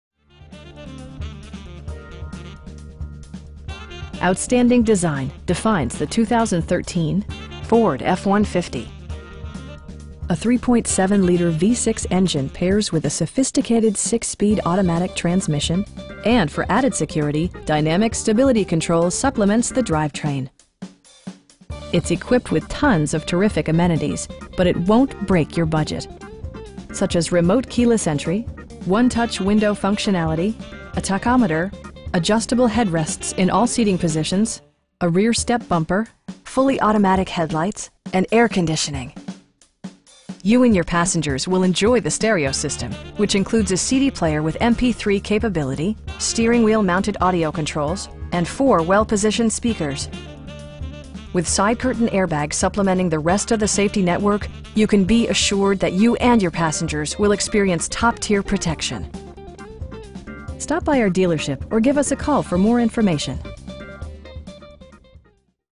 2013 Ford F-150 XLT in Mesquite, TX 75150
Outstanding design defines the 2013 Ford F-150. A 3.7 liter V-6 engine pairs with a sophisticated 6 speed automatic transmission, and for added security, dynamic Stability Control supplements the drivetrain. It's equipped with tons of terrific amenities, but it won't break your budget. Such as remote keyless entry, 1-touch window functionality, a tachometer, adjustable headrests in all seating positions, a rear step bumper, fully automatic headlights, power door mirrors, and air conditioning. You and your passengers will enjoy the stereo system, which includes a CD player with MP3 capability, steering wheel mounted audio controls, and 4 speakers, providing excellent sound throughout the cabin. With side curtain airbags supplementing the rest of the safety network, you can be assured that you and your passengers will experience top-tier protection. Our aim is to provide our customers with the best prices and service at all times. Stop by our dealership or give us a call for more information.